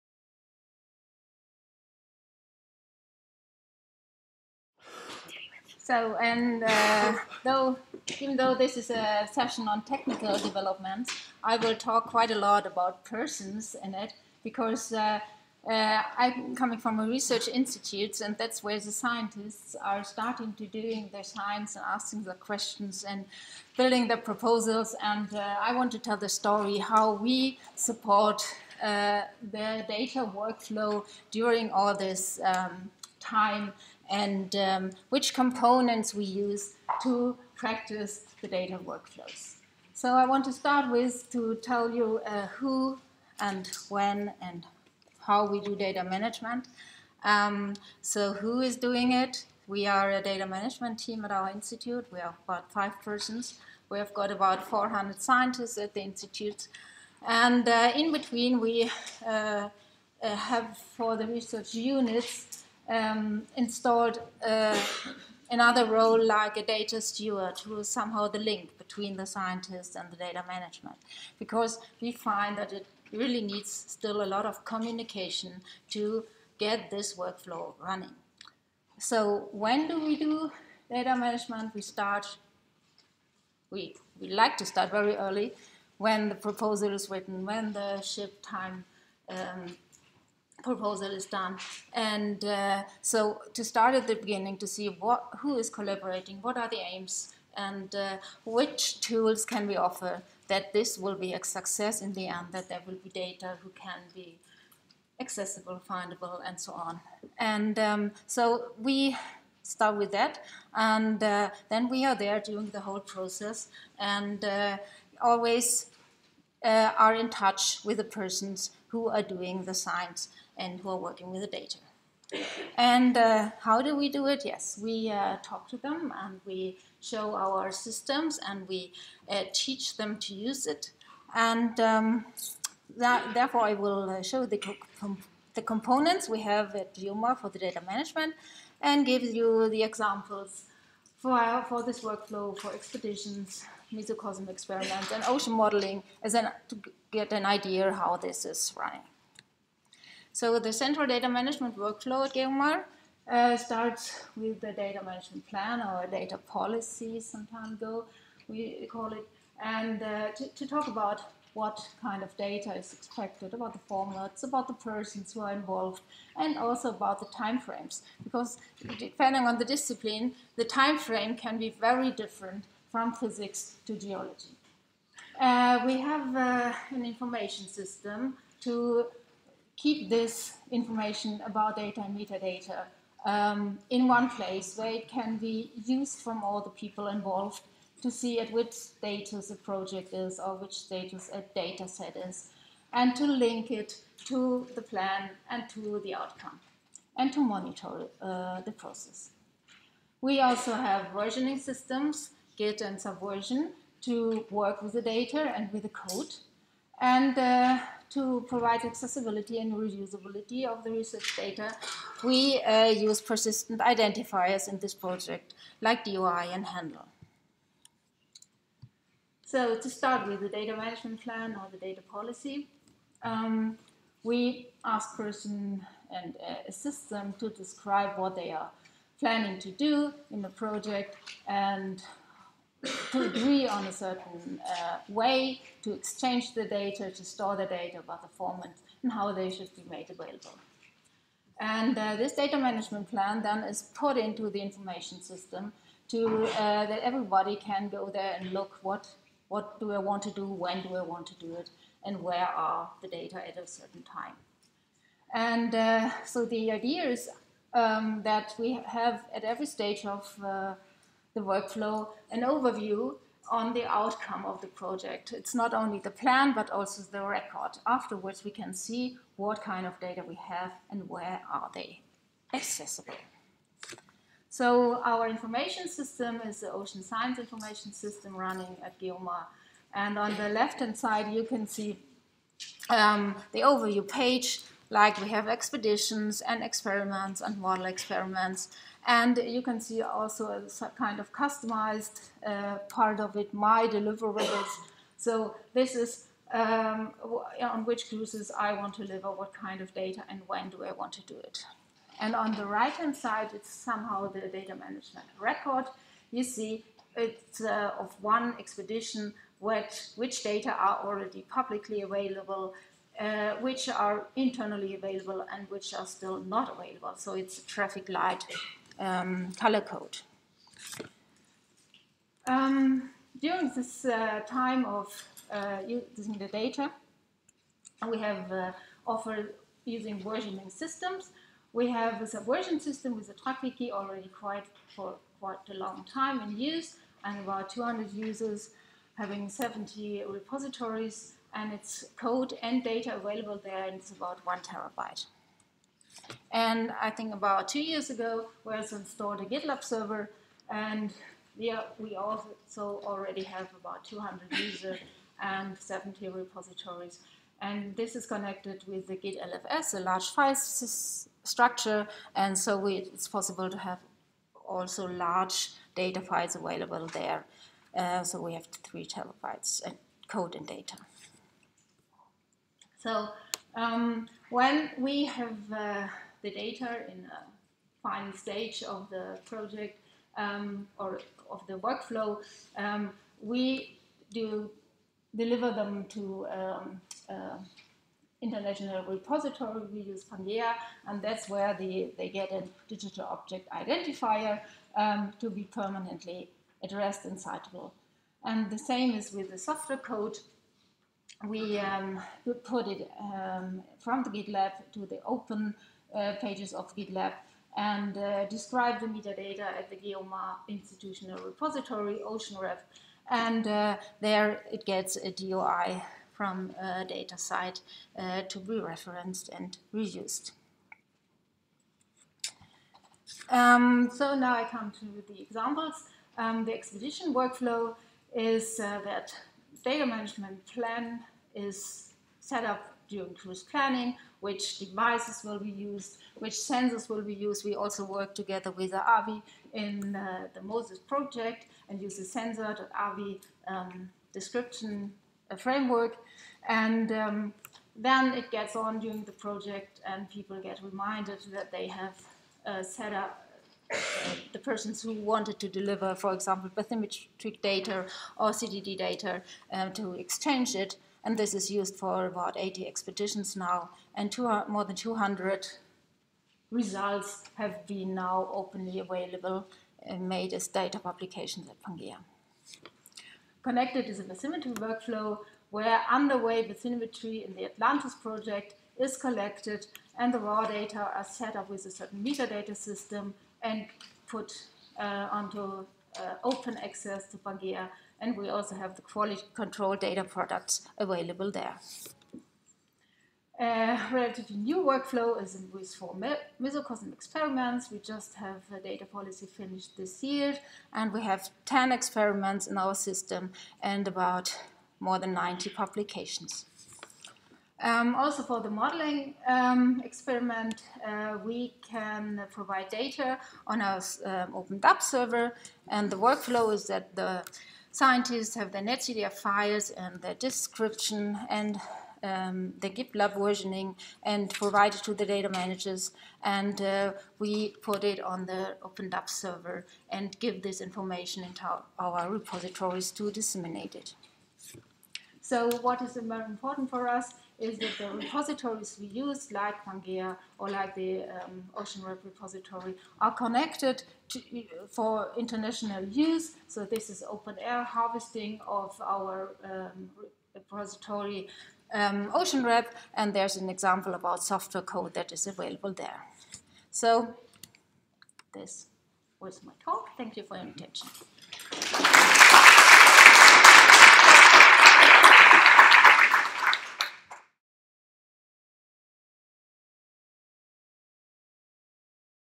IMDIS 2018 - Establishing "Best practice" at GEOMAR - Hela Mehrtens
At the International Conference on Marine Data and Information Systems 2018, 5-7 November, Barcelona.